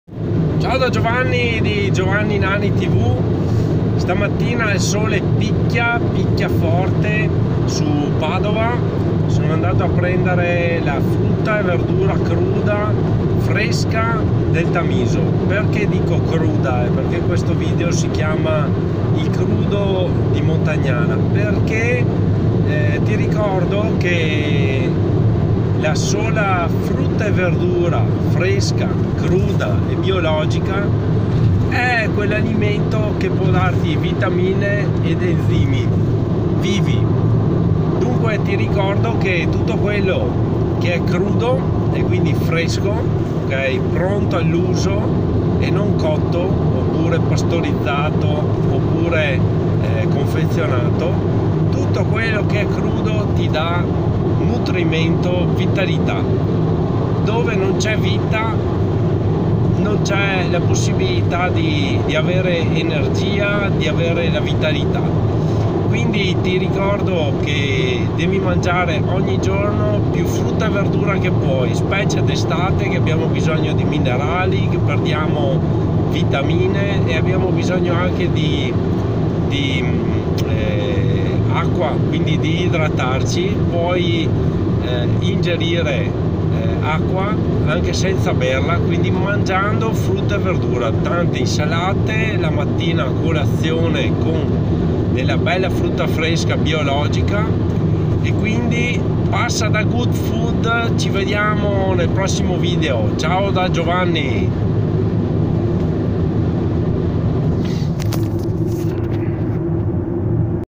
Il crudo di Montagnana lo trovi da Good food il negozio biologico
Passa al negozio biologico Good Food a fare scorta del crudo di Montagnana, frutta e verdura fresca e biologica ti aspettano!